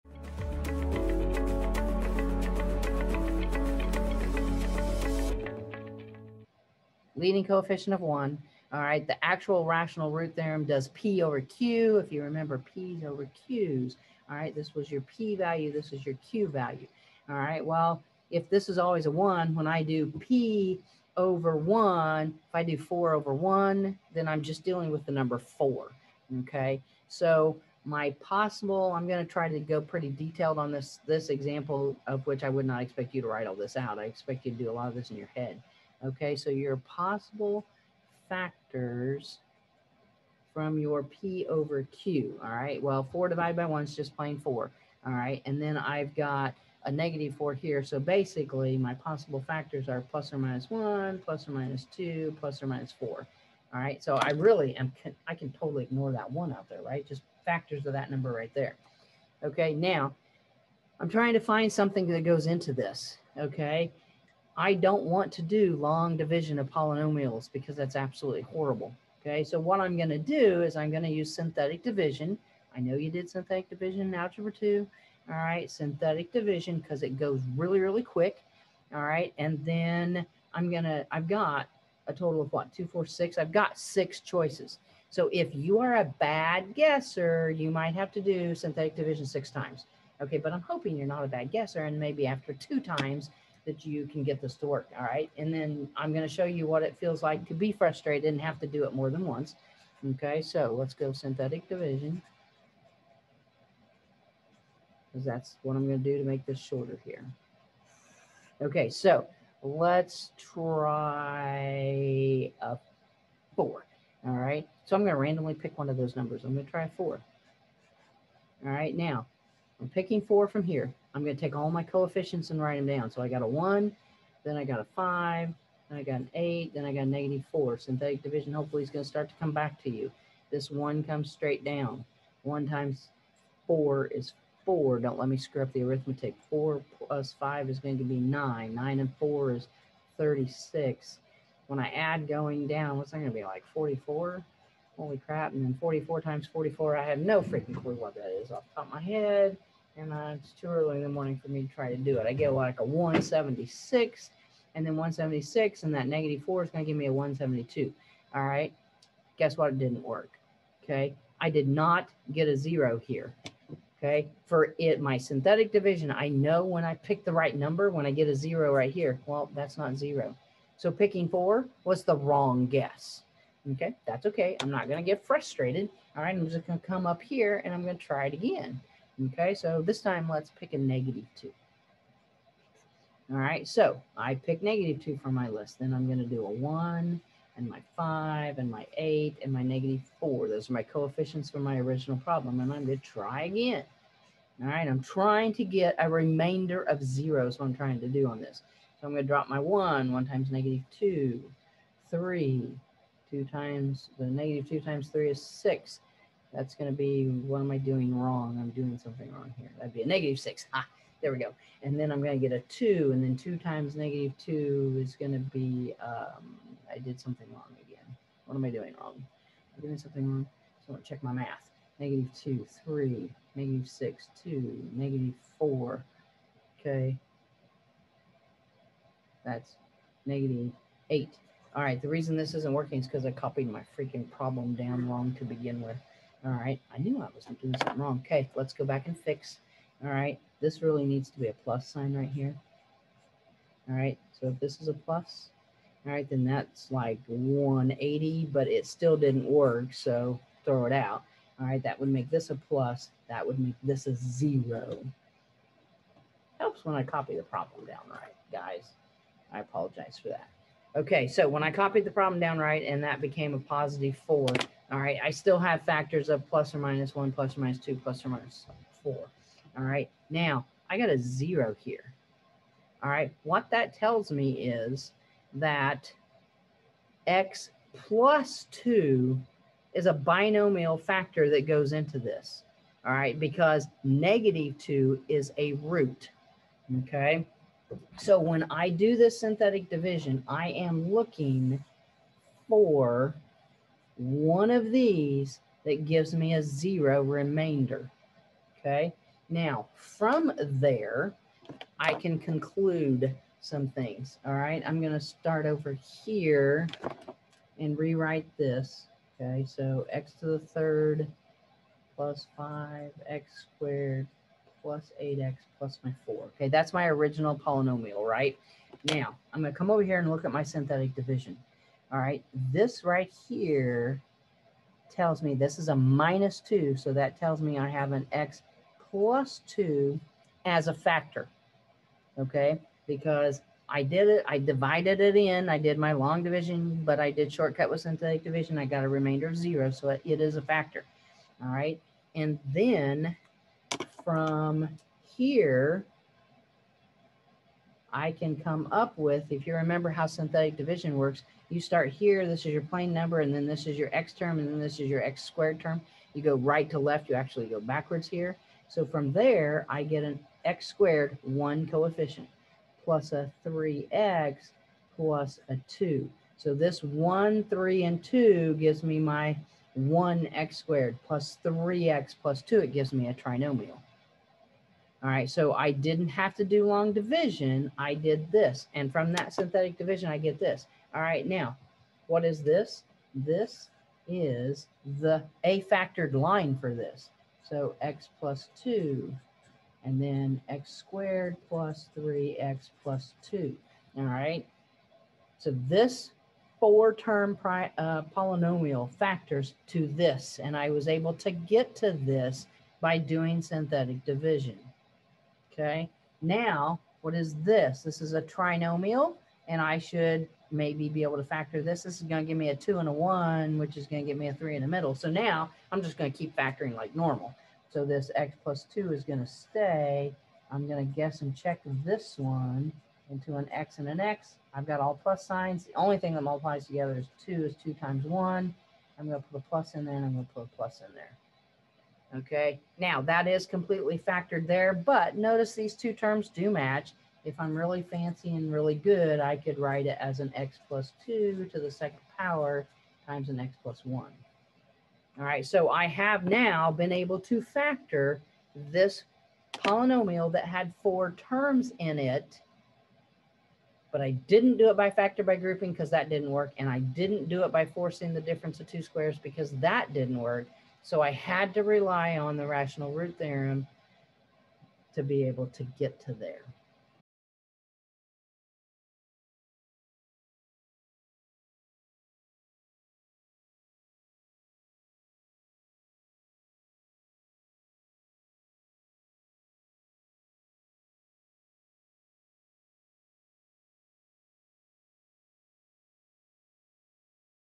Factoring with the Rational Root Theorem
This video goes through one example of how to factor a polynomial using the Rational Root Theorem.  This would typically be taught in an Algebra 2 class or a Pre-Calculus class.